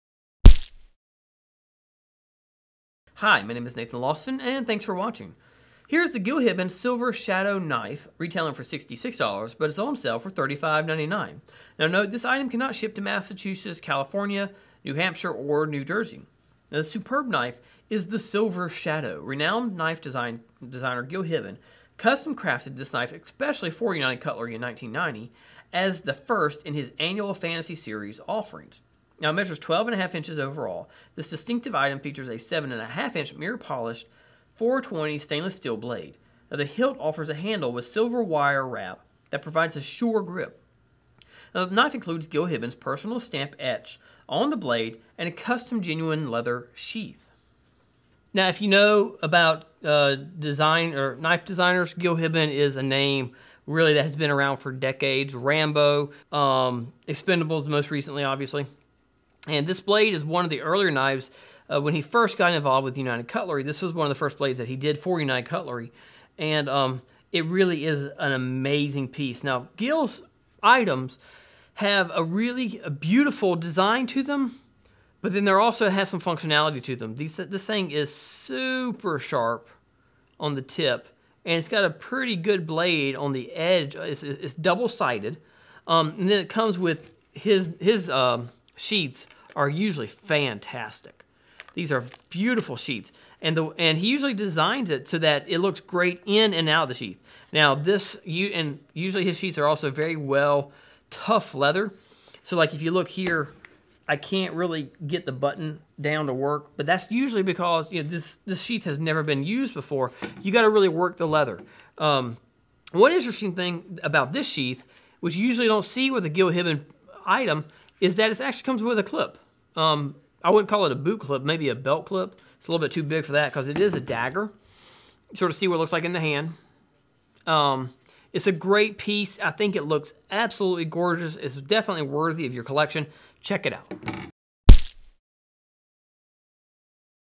Gil Hibben Silver Shadow Knife
Note: This item cannot ship to MA,CA,NH,NJ
This superb knife is the Silver Shadow. Renowned knife designer Gil Hibben custom crafted this knife especially for United Cutlery in 1990 as the first in his annual Fantasy Series offerings. Measuring 12 ½ "overall, this distinctive item features a 7 ½" mirror polished, 420 stainless steel blade. The hilt offers a handle with silver wire wrap that provides a sure grip. This knife includes Gil Hibben's personal stamp etched on the blade and a custom, genuine leather sheath. United Cutlery, GH0441.